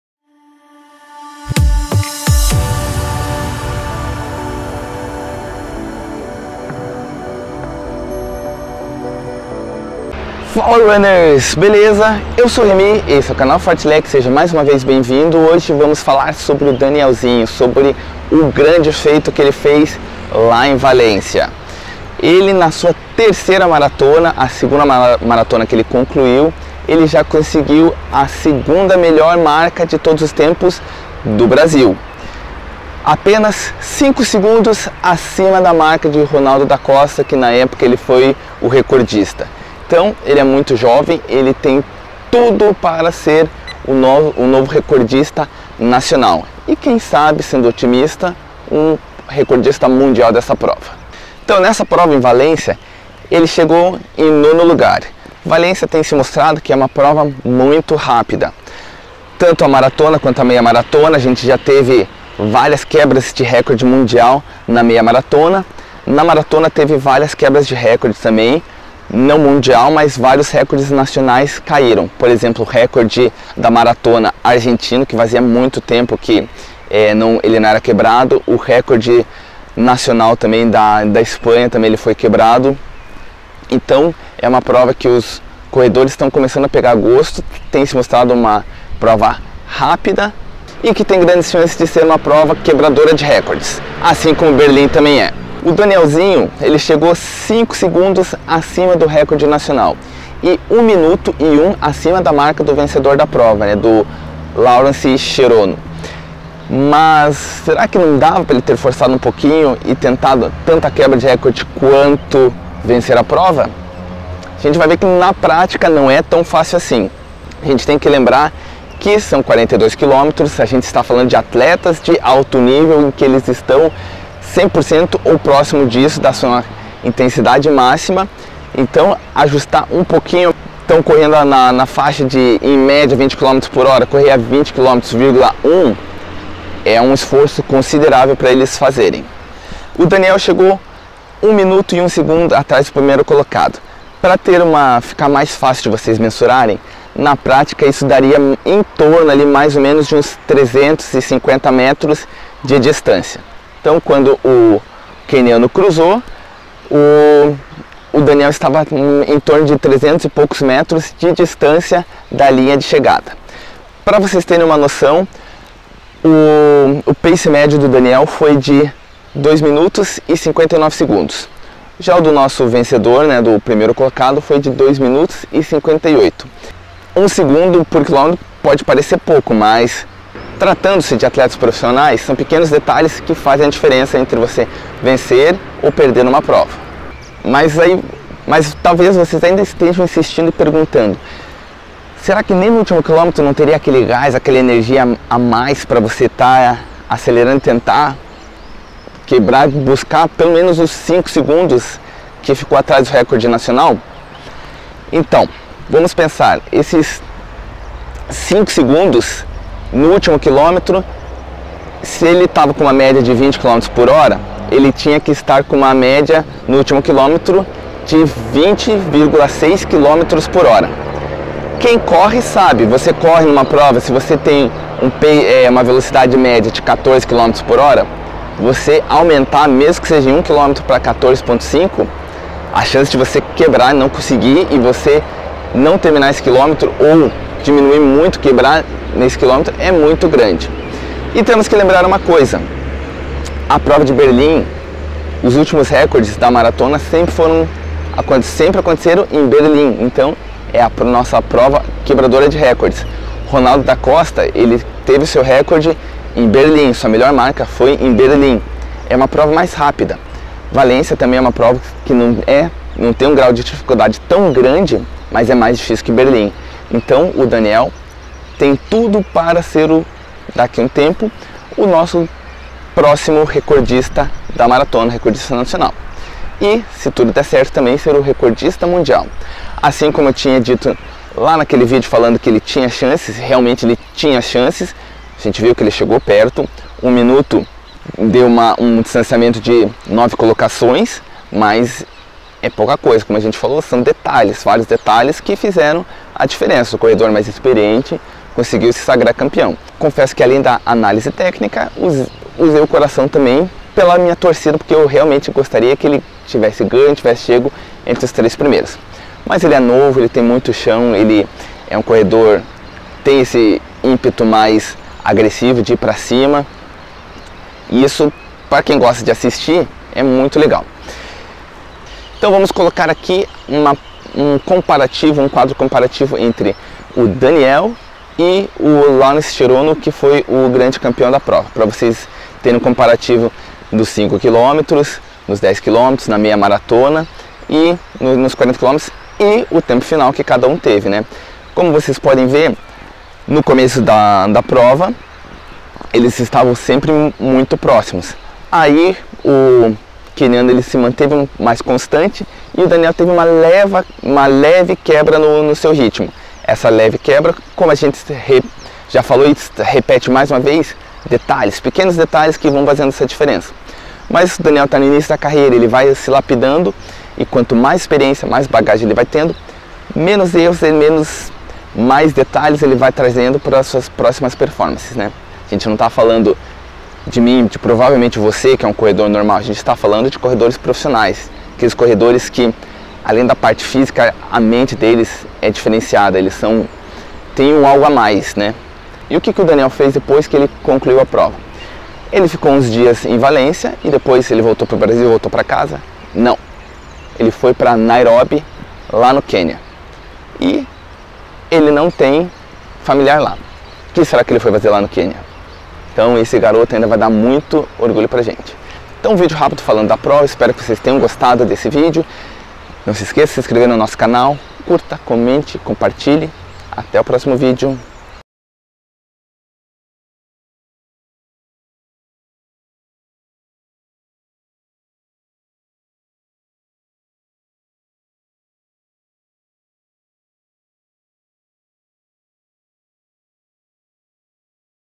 O DESEMPENHO DE DANIEL EM VALÊNCIA
Daniel teve uma performance formidável em Valência, com 2h06'11", ficou muito muito perto do recorde nacional da prova. Fizemos uma breve analise de como foi essa prova.